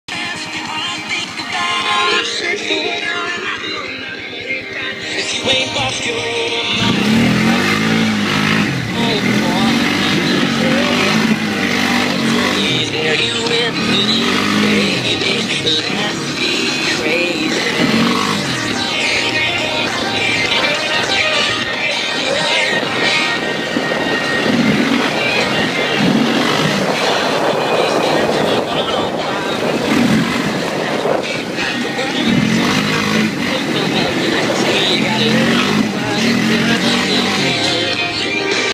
Onboard the polaris sportsman hillcimbing
Polaris sportsman 400 ho on a big ole muddy hill In southern ky